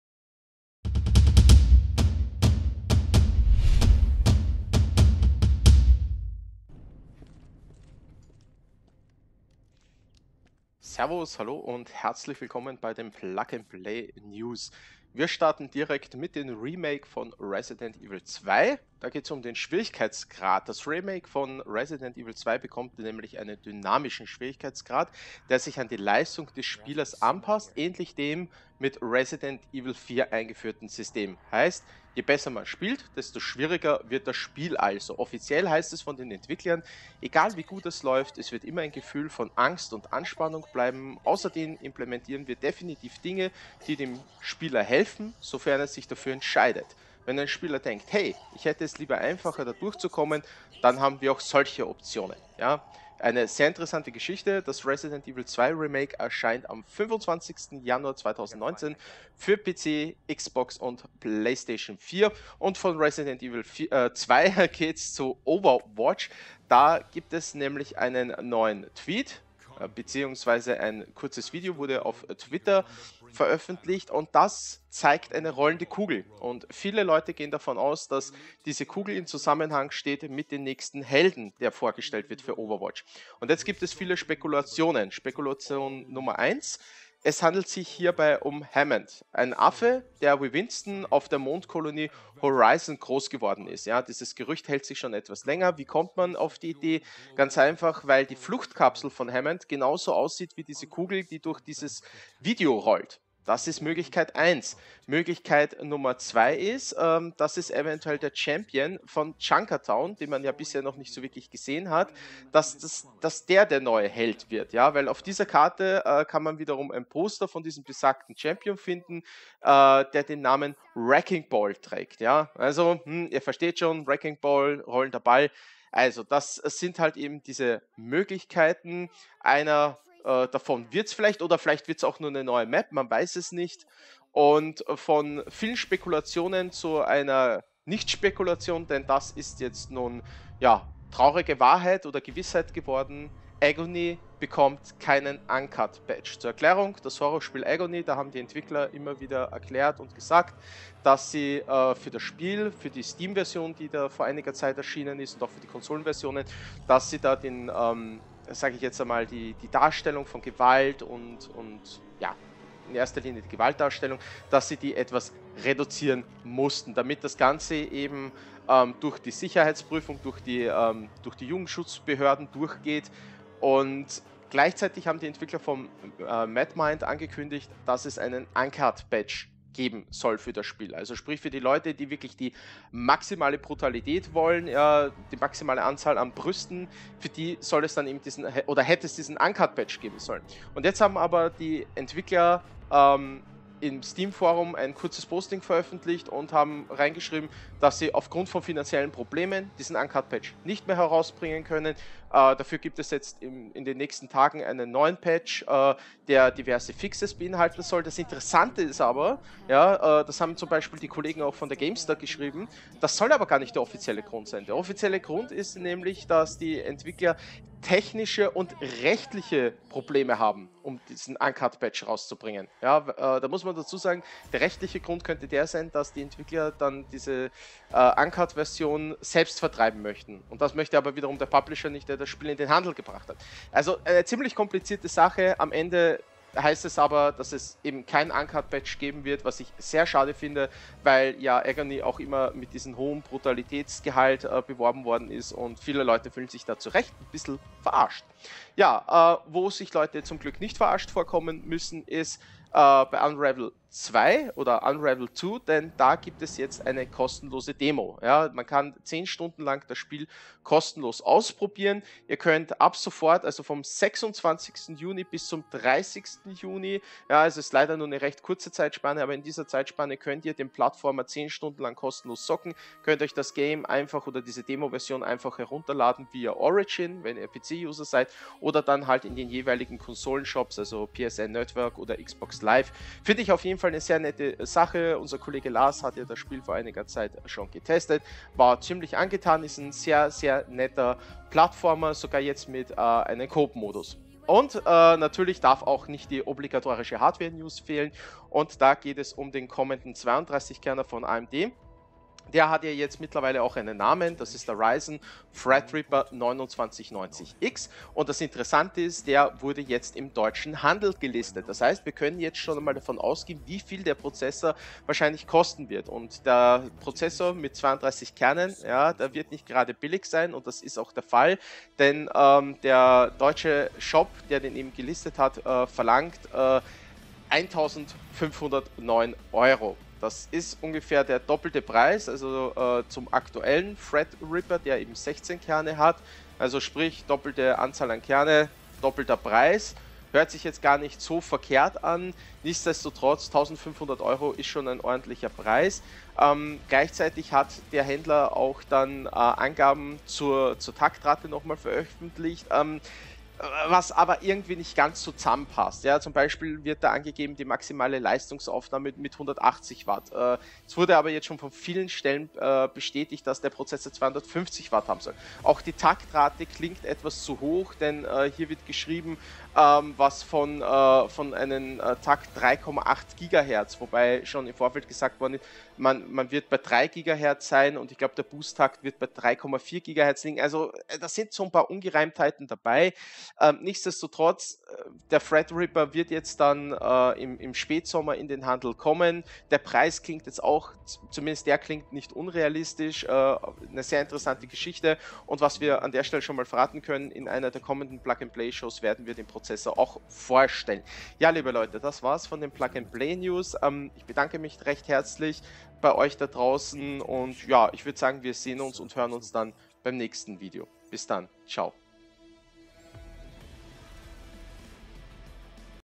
Plug and Play Episode 6: Ein neuer Held und nackte Tatsachen
Die neuesten Gaming-News der Woche auf Plug and Play: jeden Freitag um 19 Uhr auf unserem Twitch Kanal! IG Deutschland